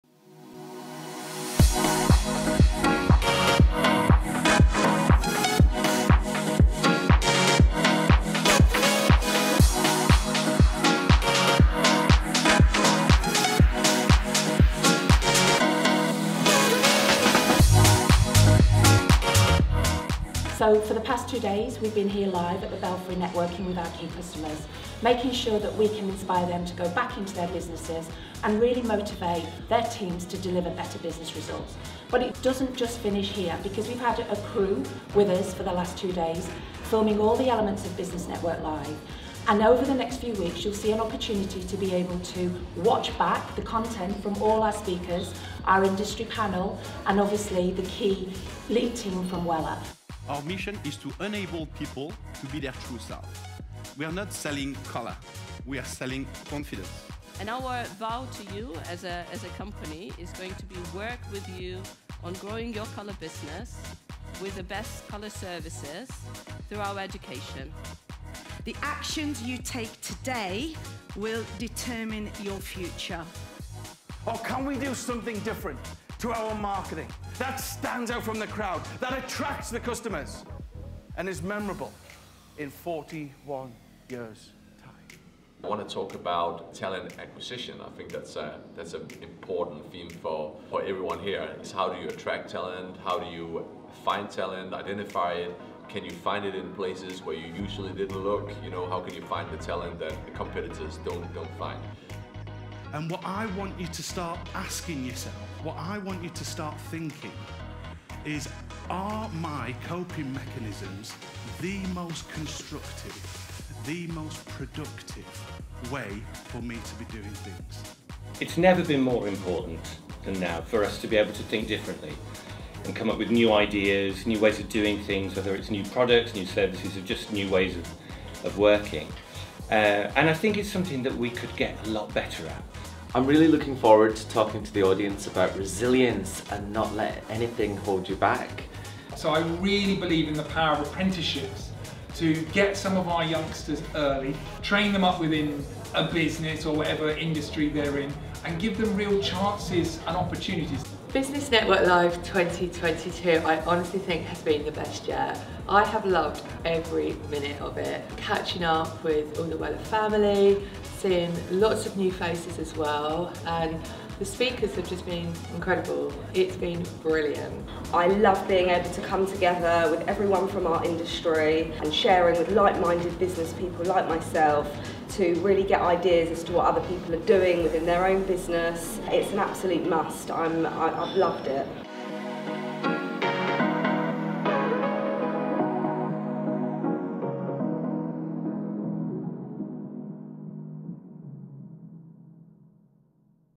Wella Business Network Live 2022 Highlights
Business Network Live has a reputation of being one of the greatest motivational events in the hairdressing calendar; the 2022 event was no exception. More than 200 guests gathered for the two-day programme filled with a fantastic line-up of motivational speakers. The inspiring content captivated the audience with real-life stories that could be adapted into invaluable business advice, empowering attendees to keep growing their businesses throughout the year.